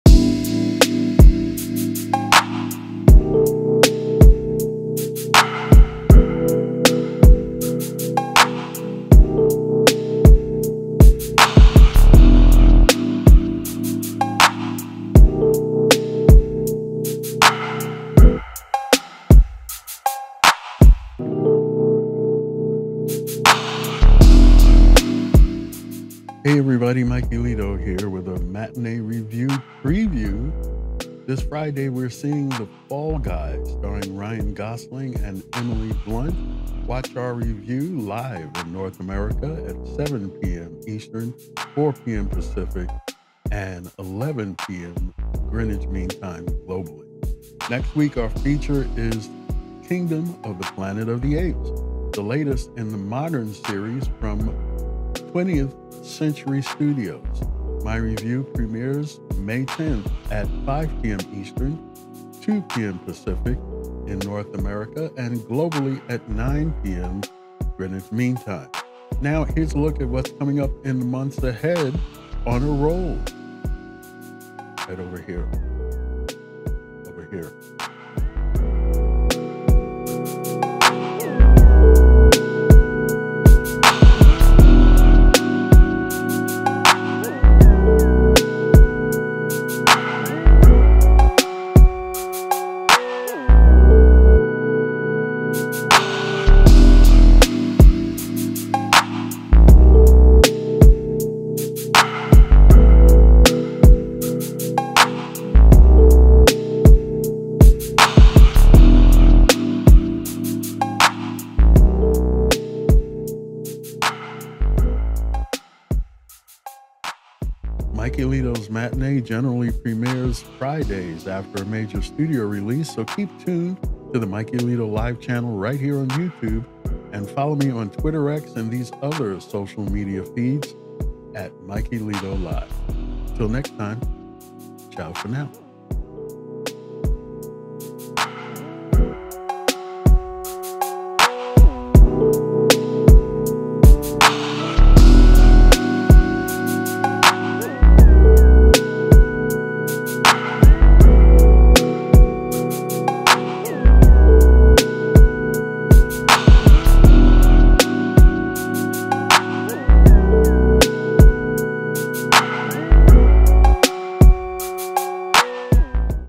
MATINÉE REVIEW PREVIEW FOR W/E 2024-05-05 | Mikeylito's Matinée
Hello, again, everybody.
I'm trying something new.

This week we're previewing the following features:
• THE FALL GUY
• KINGDOM OF THE PLANET OF THE APES
• More...

I'll also list which summer films I plan on seeing this summer.
Follow @MikeylitoLIVE on TwitterX for the latest programming updates.